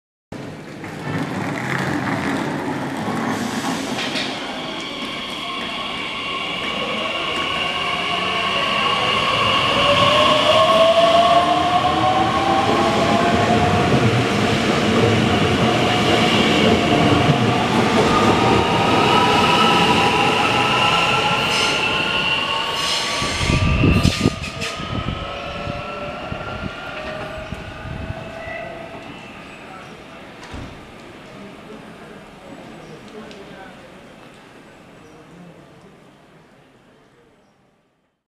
Abfahrt RB48 Wuppertal Hbf
Abfahrt einer Regionalbahn von National Express in Richtung Köln.